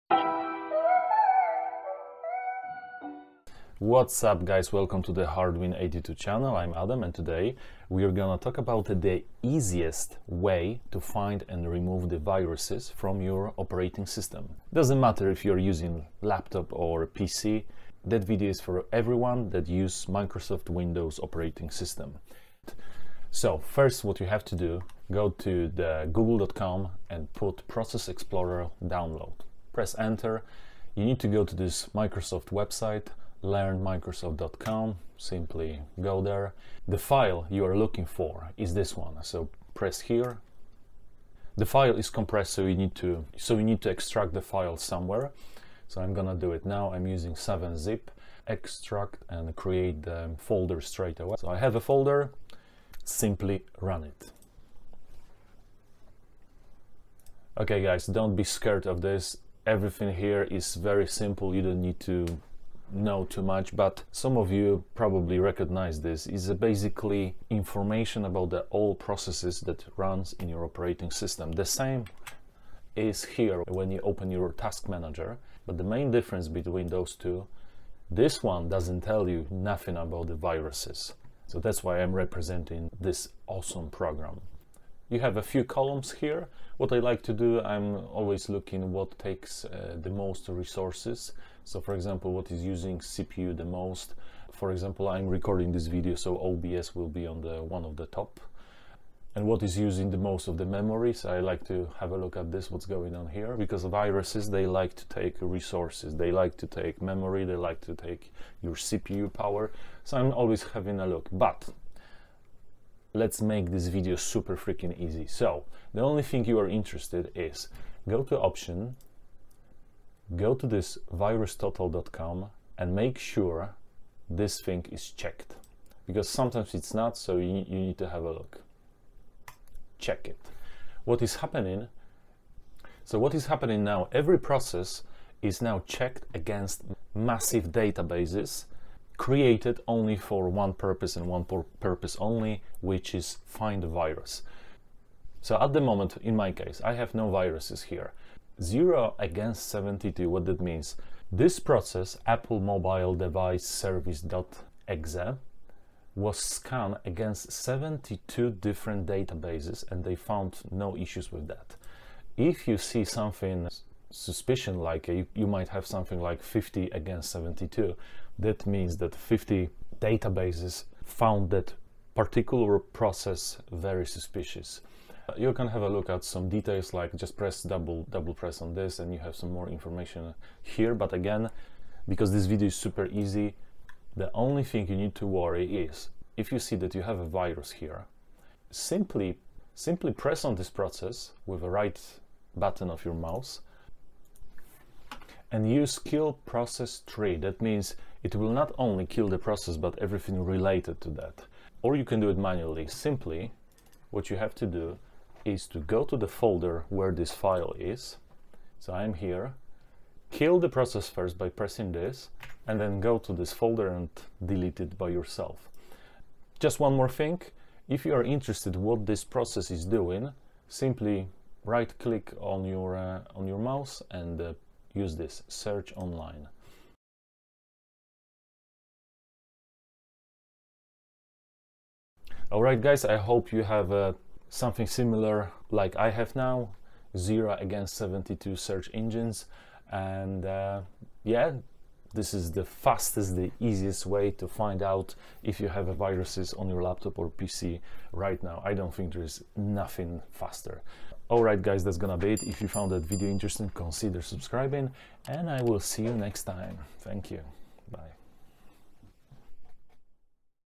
Remove any virus from your computer or laptop fast and free 2024
The easiest and fastest way to remove any viruses, malware, or trojans from your computer or laptop with the windows operating system out there.

and check it right now.